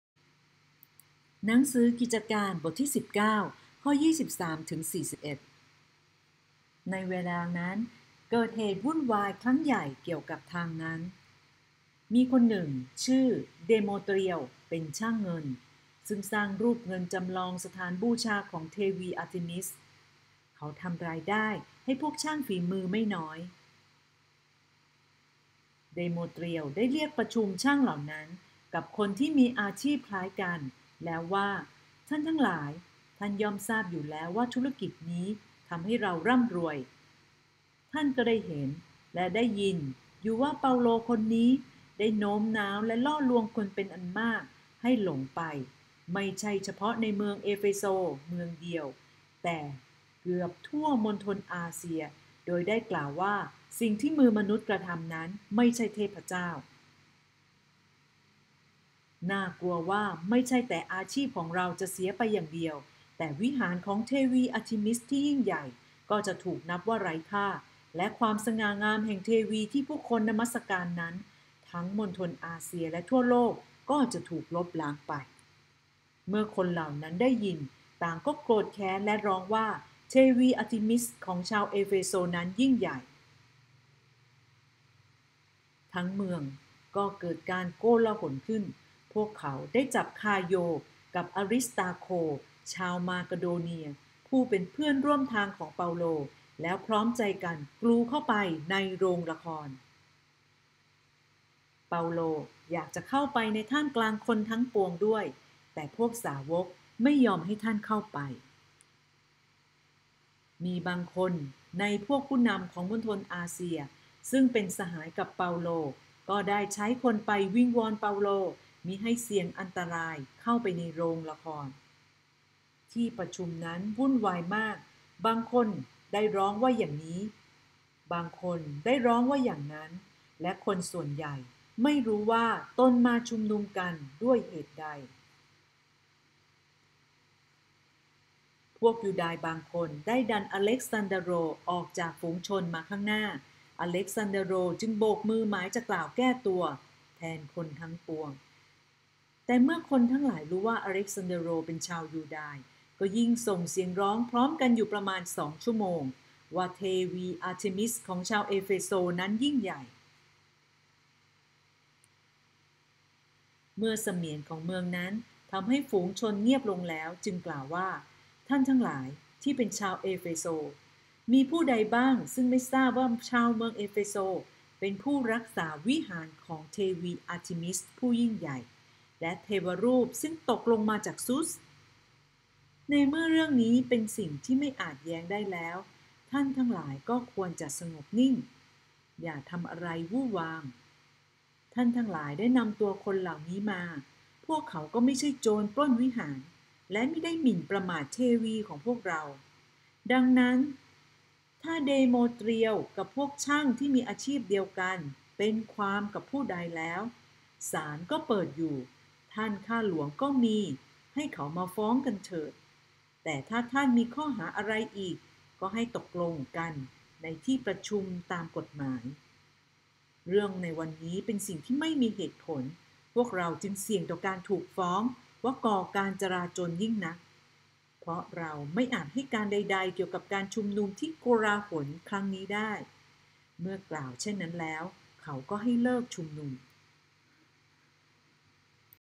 กิจการ 19ข้อ 23 ถึง 41 ช่างเงิน ใน เมือง เอเฟโซ ลุกขึ้นมาต่อต้าน เปาโล เทวี ช่างเงิน วิหารเทวี
กิจการ 19ข้อ 23 ถึง 41 ช่างเงิน ใน เมือง เอเฟโซ ลุกขึ้นมาต่อต้าน เปาโลช่างเงิน วิหารเทวี